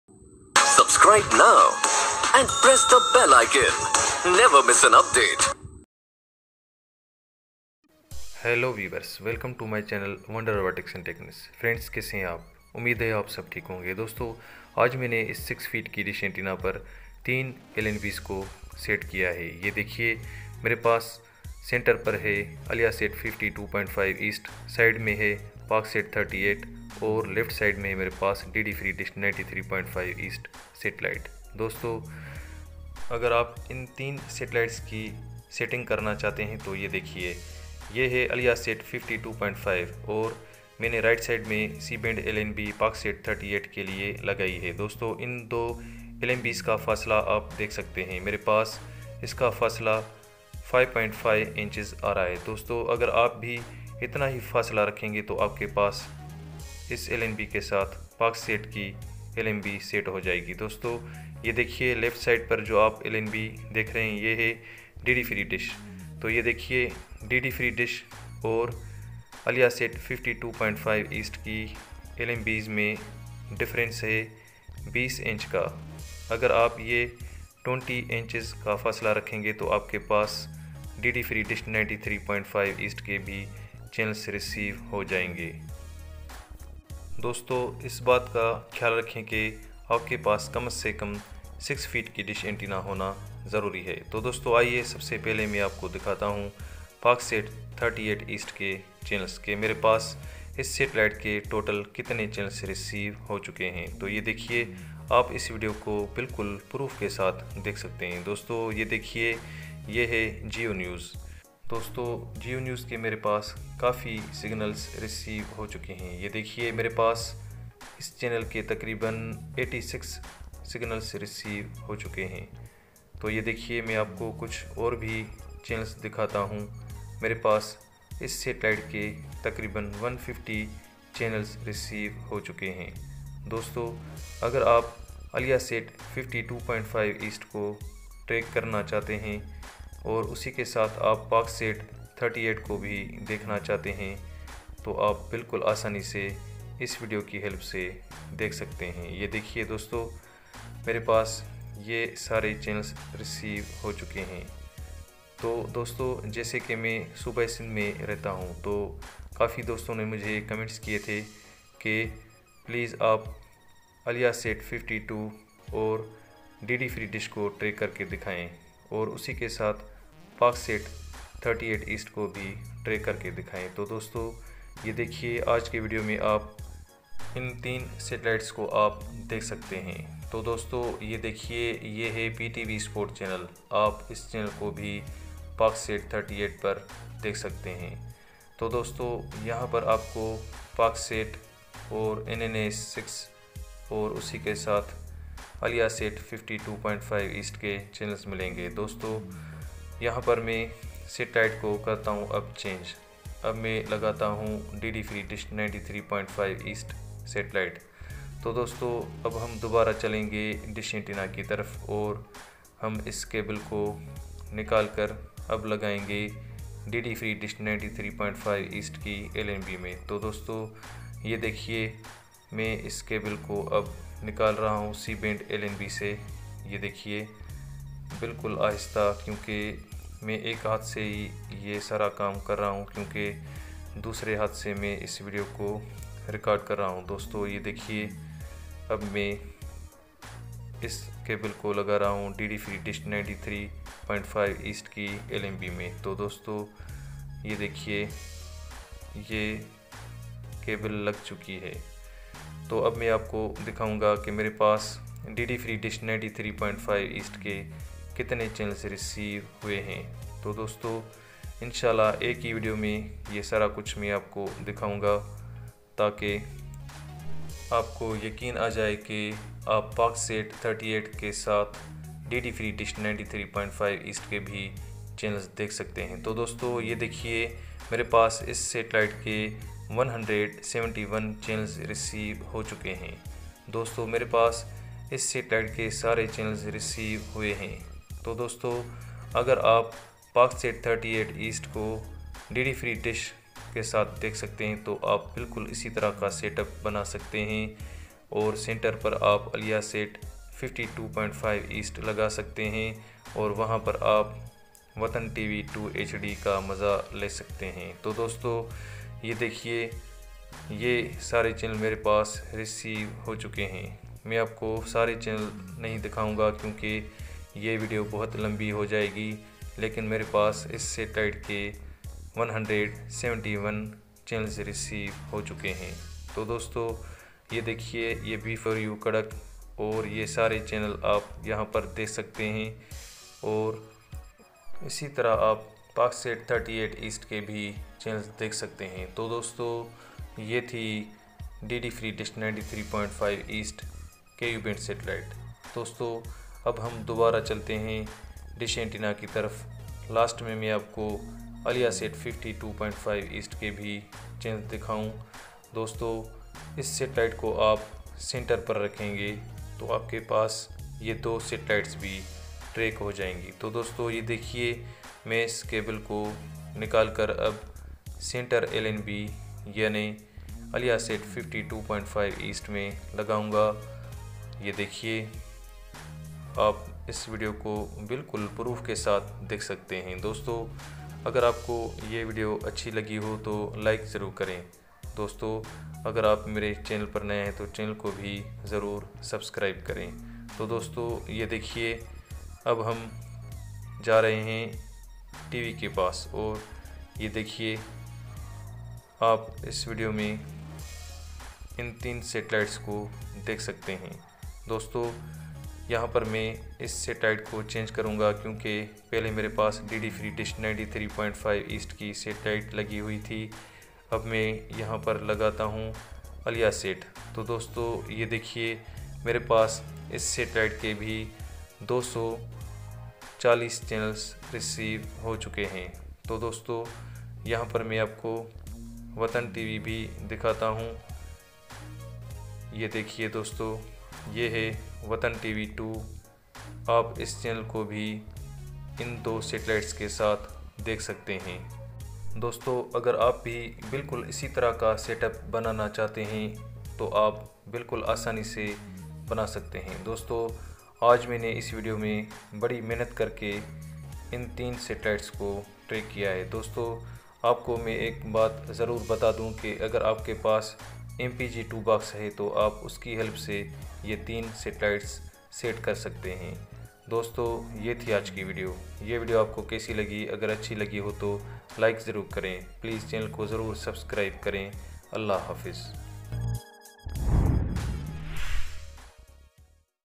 Paksat @38e, Watan 2 on Al-Yahsat @52.5e & DD free dish @93.5e satellite all channels on one dish.
Hi, dear friends! This video is about three best satellites. Paksat @38e, Watan 2 on Al-Yahsat @52.5e & DD free dish @93.5e satellite all channels on one dish antenna. You can see complete setting in this video. Thanks